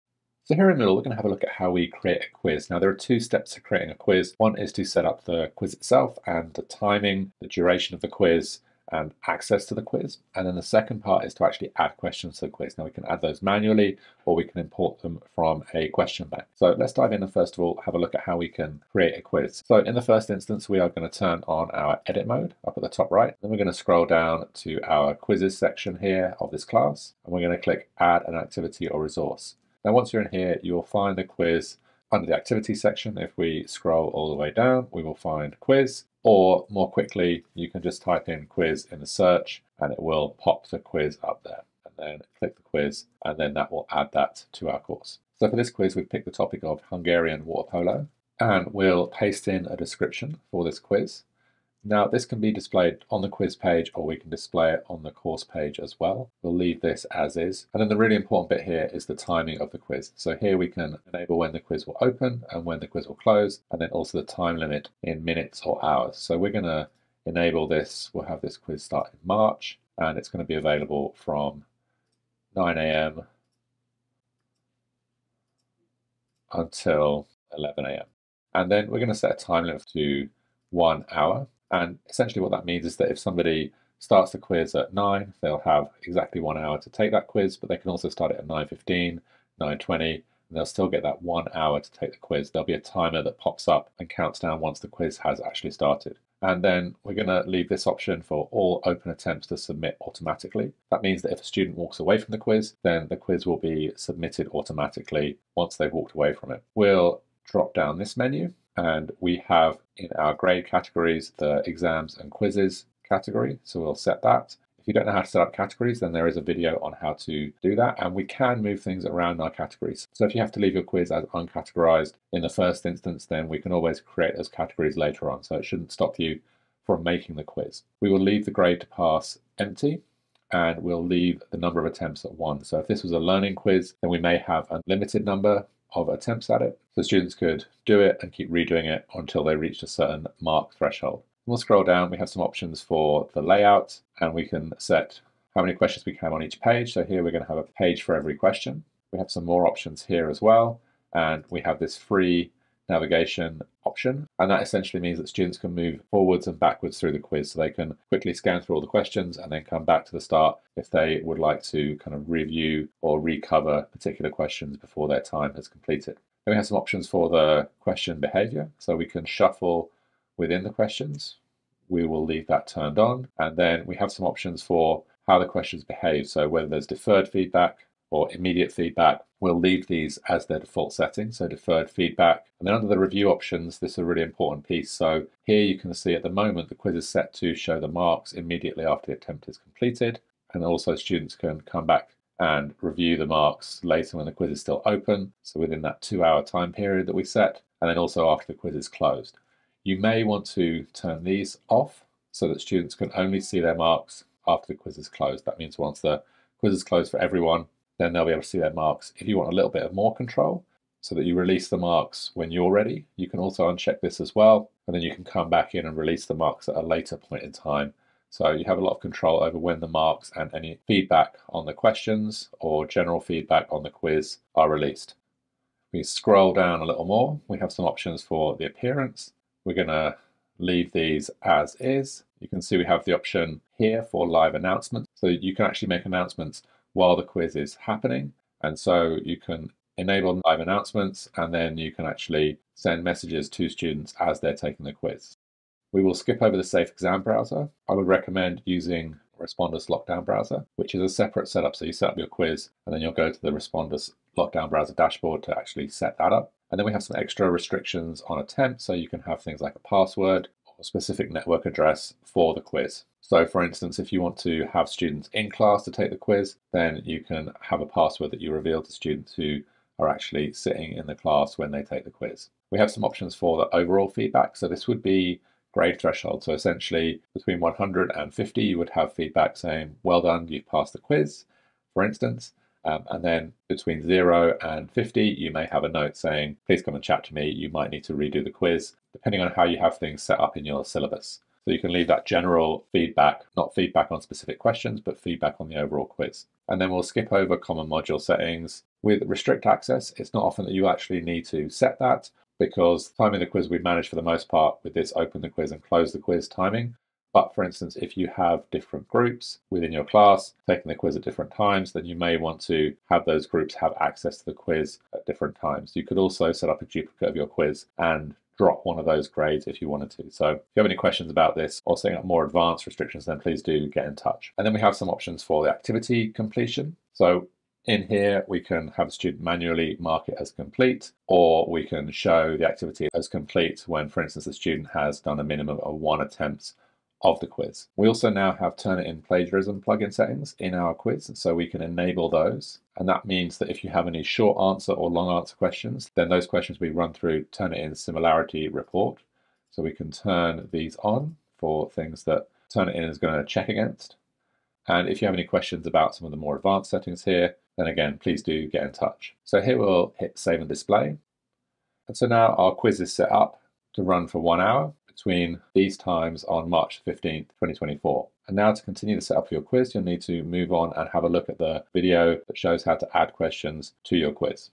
Creating a Quiz in Moodle 4.1 - Part 01 - Quiz Setup
Learn how to setup your quiz in Moodle. Including quiz creation, quiz setup and useful tips for Respondus Lockdown Browser and Turnitin.